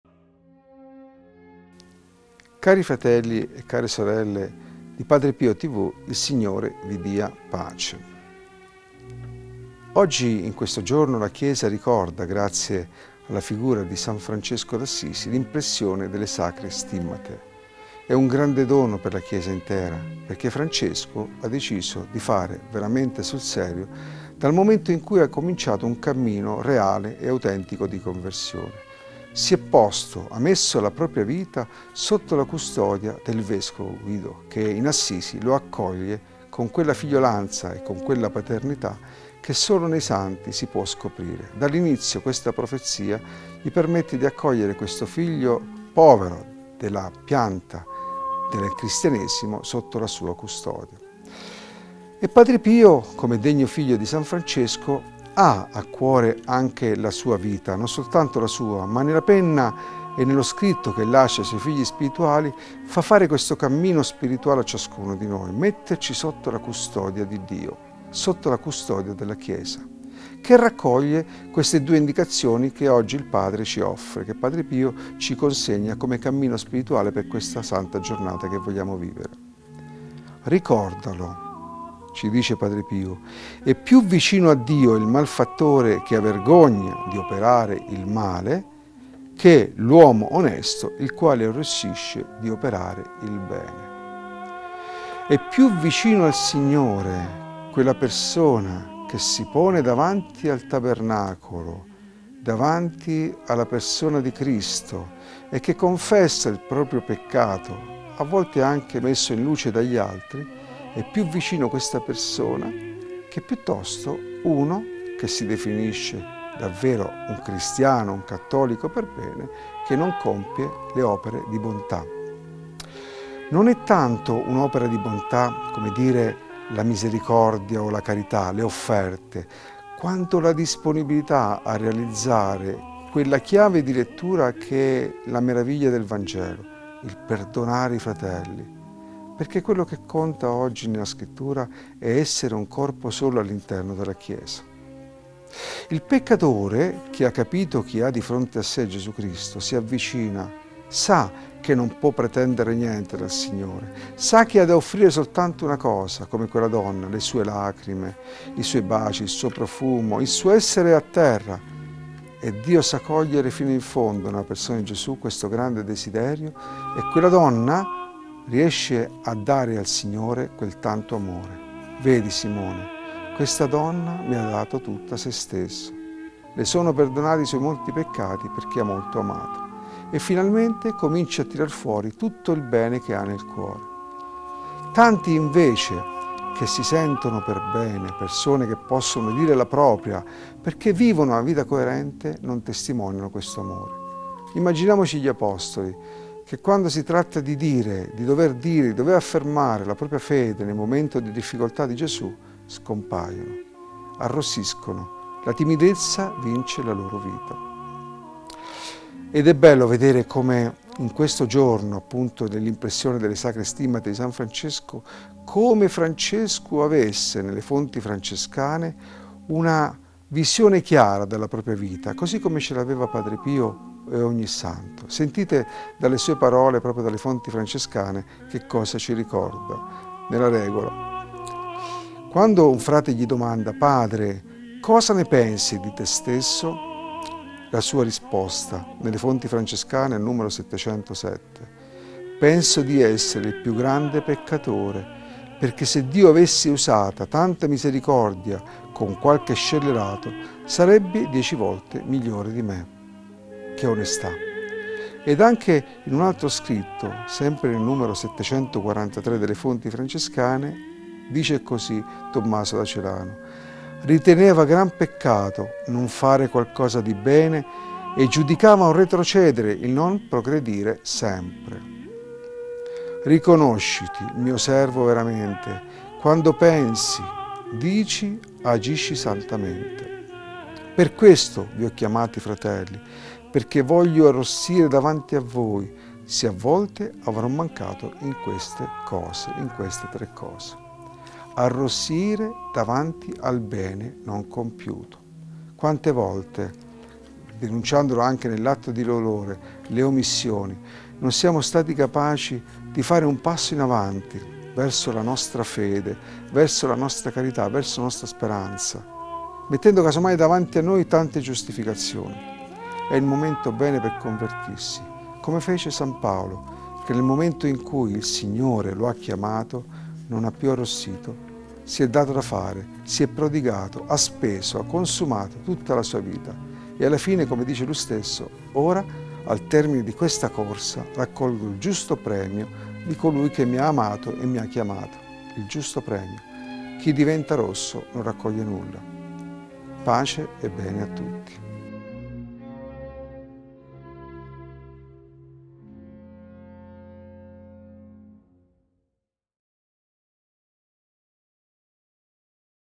Padre Pio come Francesco d'Assisi
Fr Orazio Renzetti - Buona Giornata con Padre Pio 17 Settembre 2019
Ogni giorno i Frati Cappuccini augurano la buona giornata con le parole di Padre Pio alle ore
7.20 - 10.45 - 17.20

La Frase di Padre Pio:
"Ricordalo: è piú vicino a Dio il malfattore che ha vergogna di operare il male che l’uomo onesto il quale arrossisce di operare il bene".